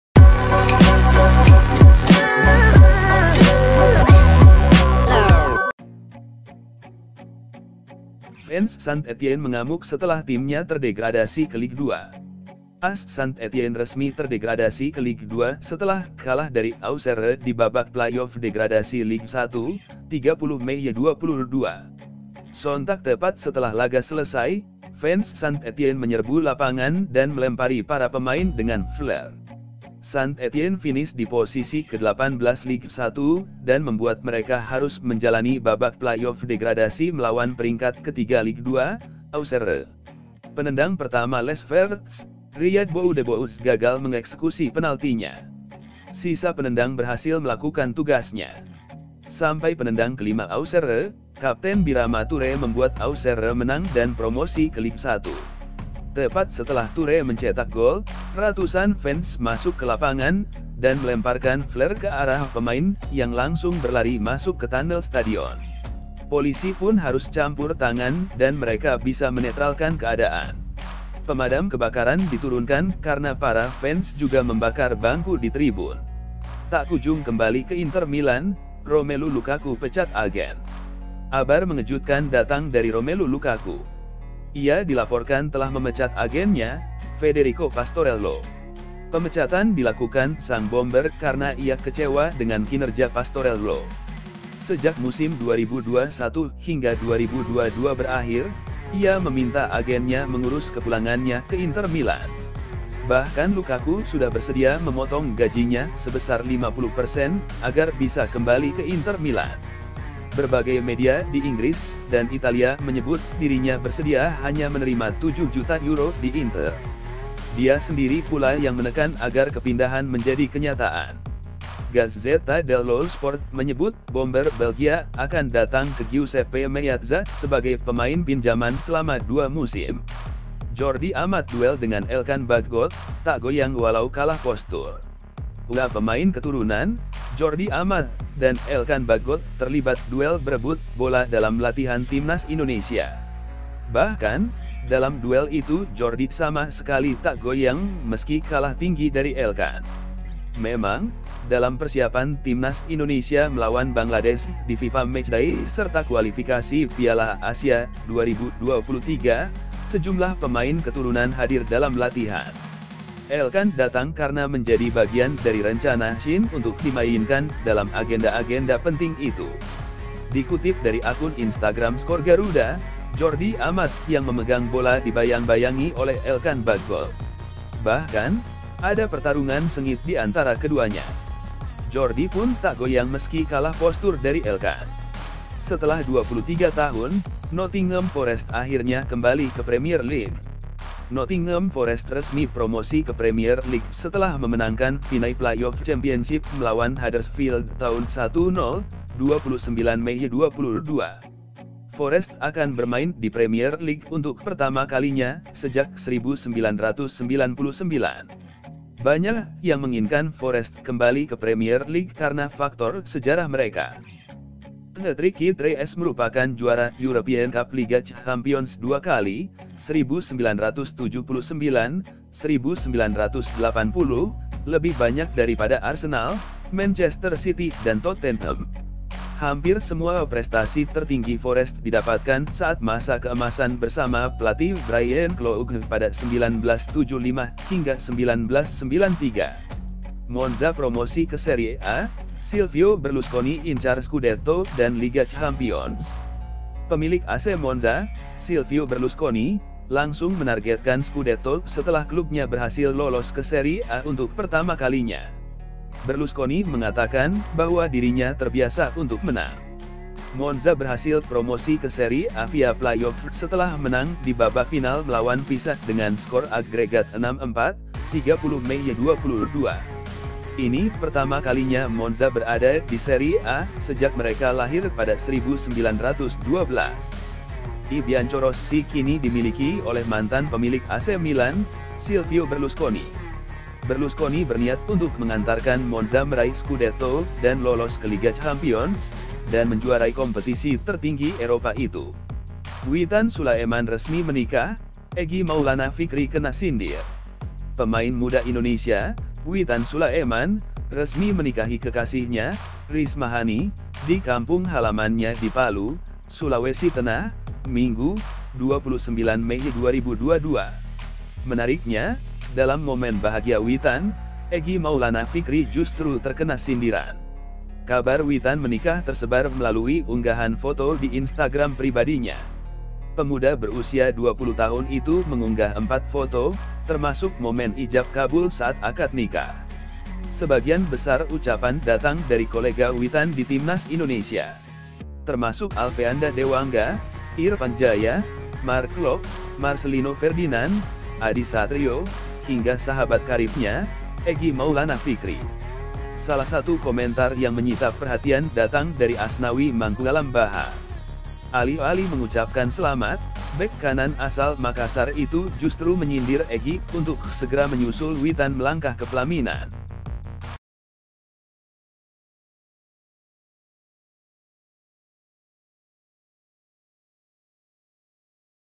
BERITA SEPAK BOLA : "DUEL ELKAN BAGGOTT VS JORDI AMAT😎, FANS SAINT ETIENNE NGAMUK & LUKAKU NGAMBEK"
0:00 Fans Saint-Etienne Mengamuk Setelah Timnya Terdegradasi ke Ligue 2
1:19 Tak Kujung Kembali ke Inter Milan, Romelu Lukaku Pecat Agen
2:10 Jordi Amat Duel dengan Elkan Baggott, Tak Goyang Walau Kalah Postur
3:09 Setelah 23 Tahun, Nottingham Forest Akhirnya Kembali ke Premier League
4:05 Monza Promosi ke Serie A, Silvio Berlusconi Incar Scudetto dan Liga Champions
4:59 Witan Sulaeman Resmi Menikah, Egy Maulana Vikri Kena Sindir